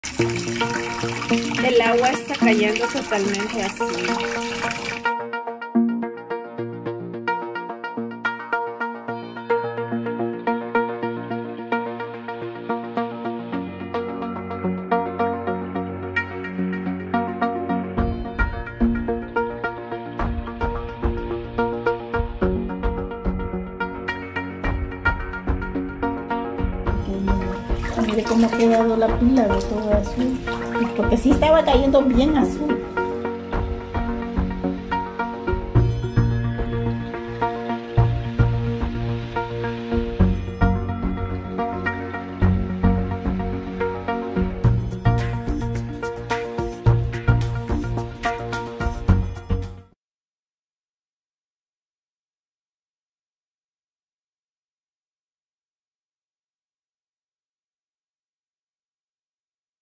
Agua de ANDA salió azul en Mejicanos
Tres familias en Mejicanos recibieron del grifo agua azulada y con un leve olor metálico, la tarde del 15 de diciembre de 2020. Los afectados son abonados de ANDA y residentes en un edificio óctuple en la colonia Zacamil (Mejicanos).